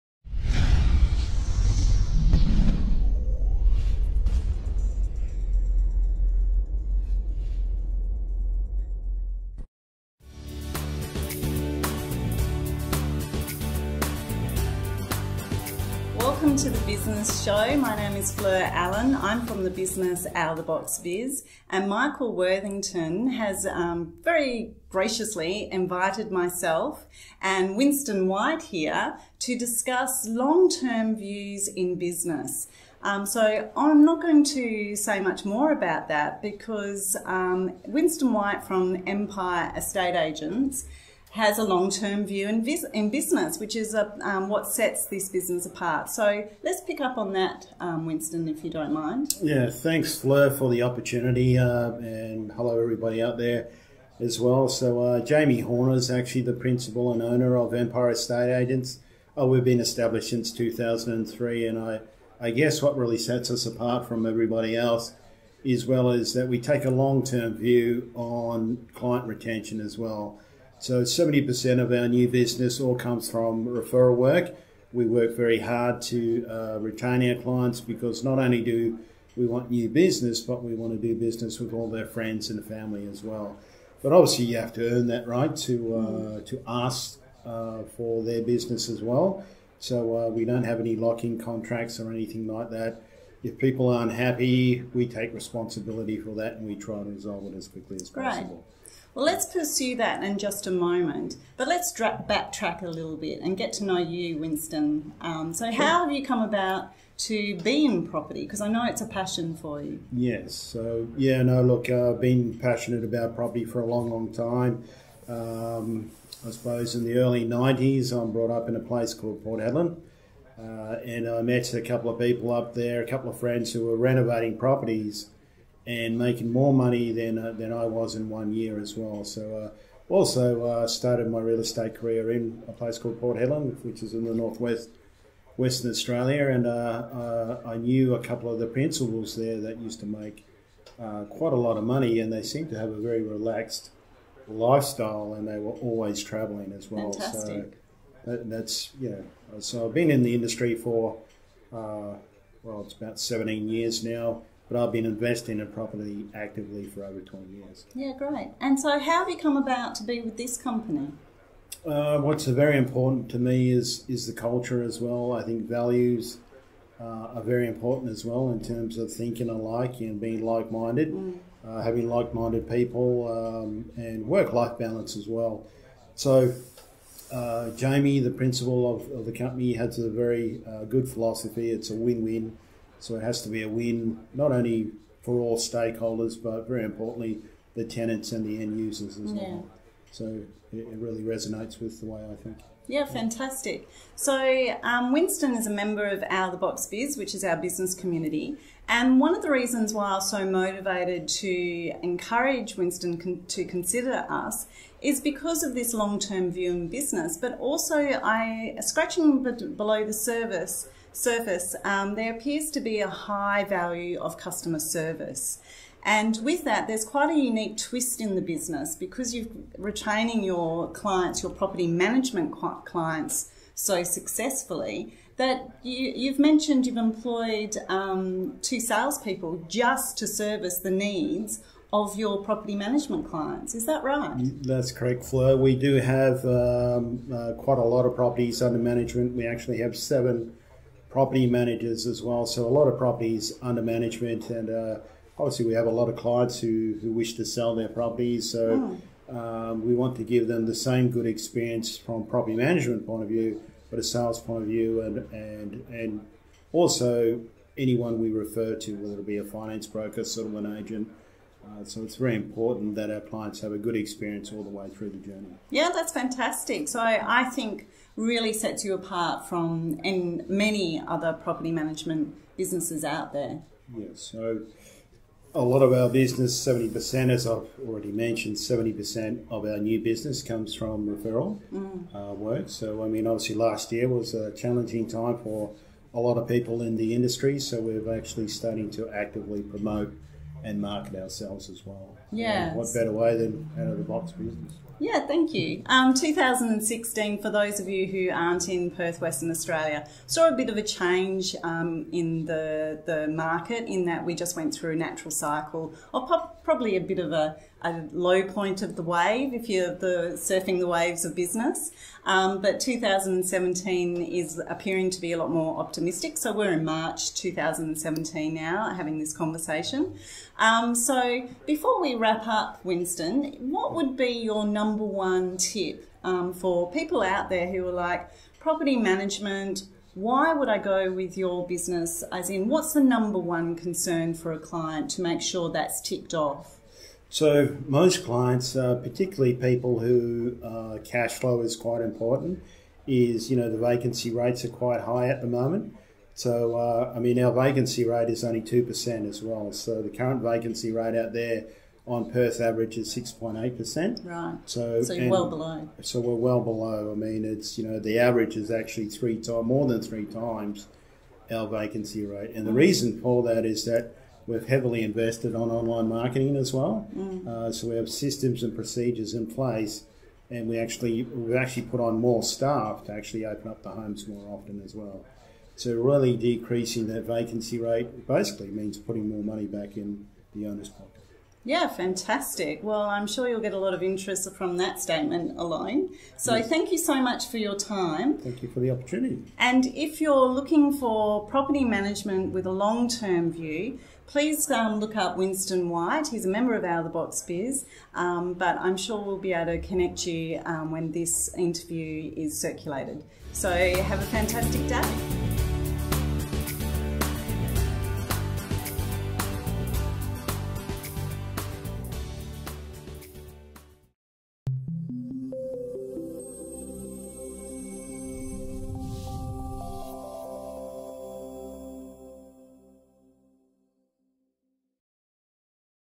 Winston White from Empire Estate Agents on Property Management in Perth
Property Management at Empire - Owners For Life

Fleur Allen from Out of the Box Biz sits with Winston White from Empire Estate Agents to get to know Winston and his life long love of property and talk about:
- Why an Owner should choose Empire to manage their property investment
- Client retention is a key focus at Empire
- Our no lock in contracts ensure we provide owners peace of mind and quality service
- Empire Property Management systems and procedures to ensure we get your property leased
- Empire property management customer service and client retention focus
- Property Management team vacancy rates

Plus more

Empire Estate Agents
966 Albany Highway
East Victoria Park WA 6101